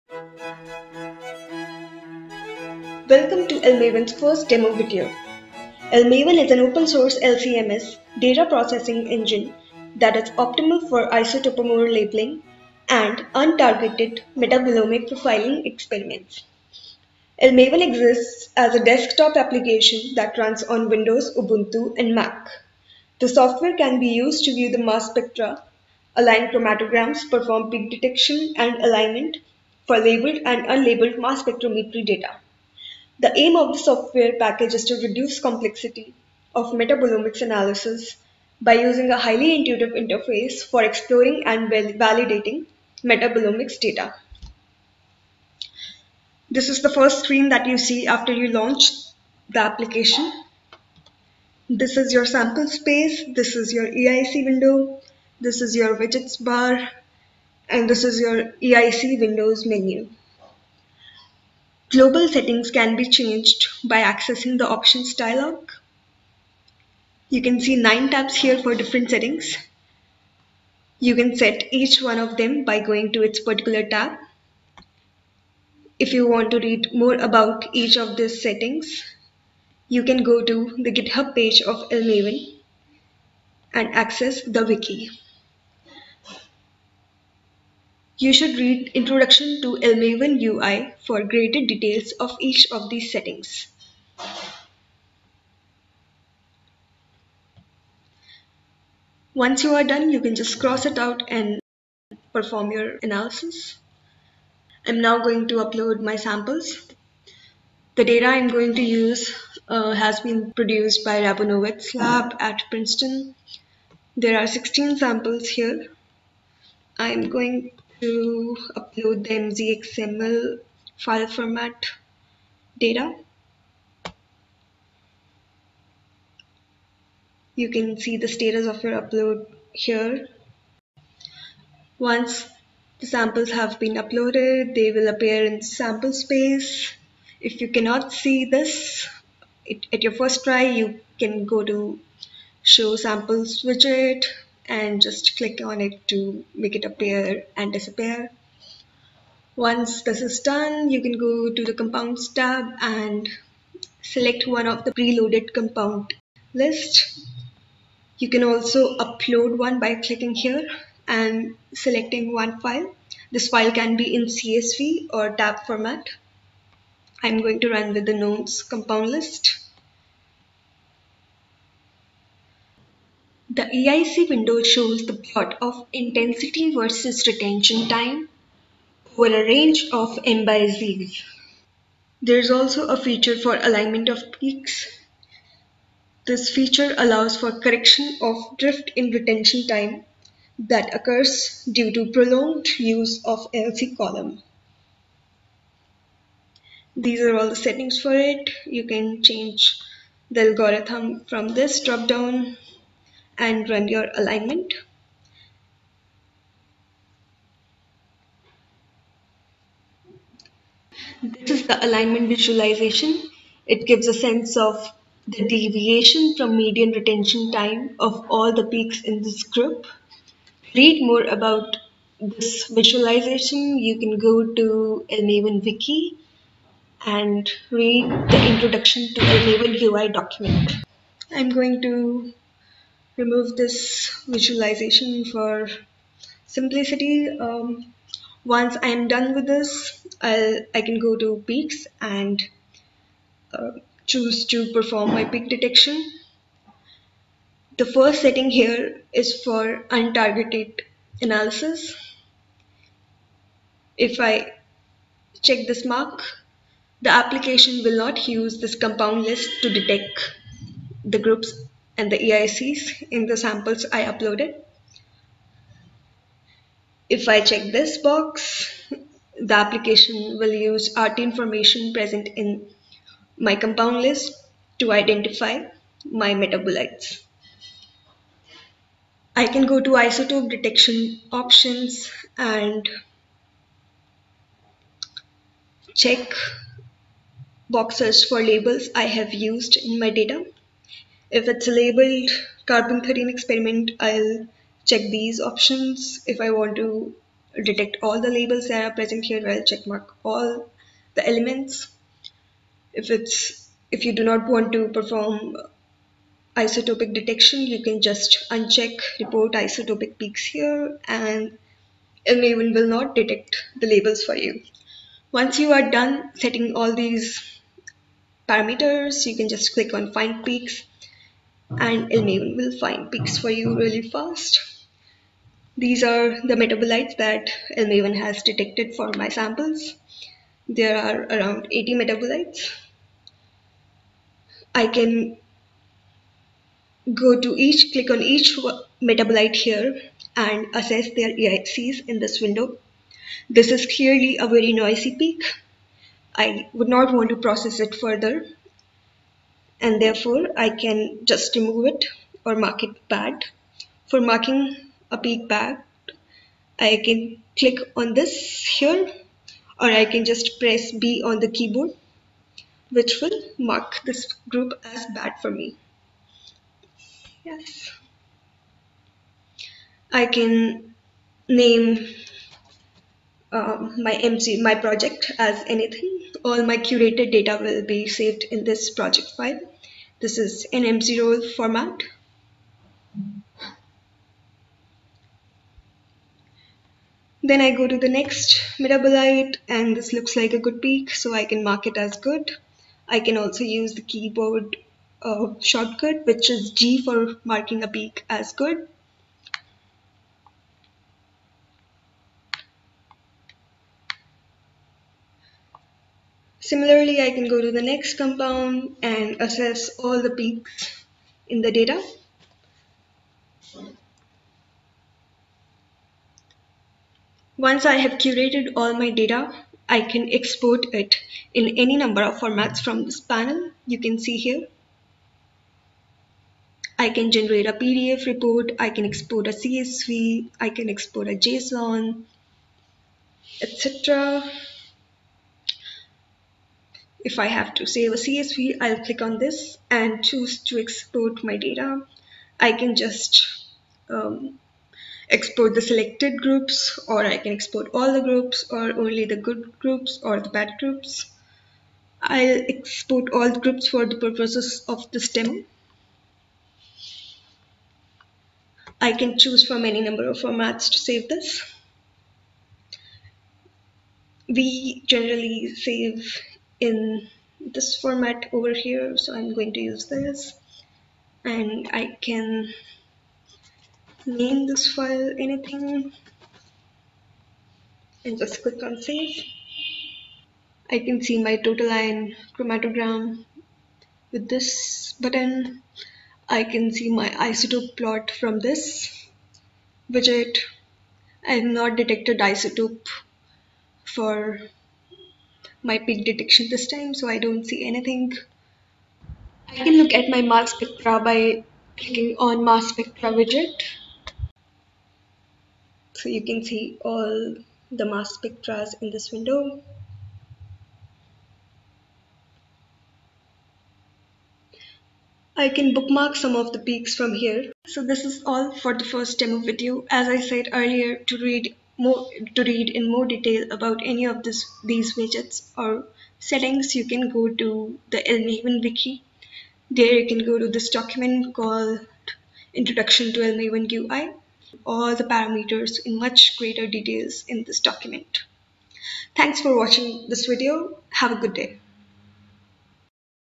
Introduction to El-MAVEN UI
El-MAVEN is an open source LC-MS and LCMS/MS raw data processing engine that is optimal for isotopomer labeling and untargeted metabolomic profiling experiments. It can be used to view the mass spectra, align chromatograms, perform peak-feature detection and alignment for labeled and unlabeled mass spectrometry data.

Don't just watch it, experience it!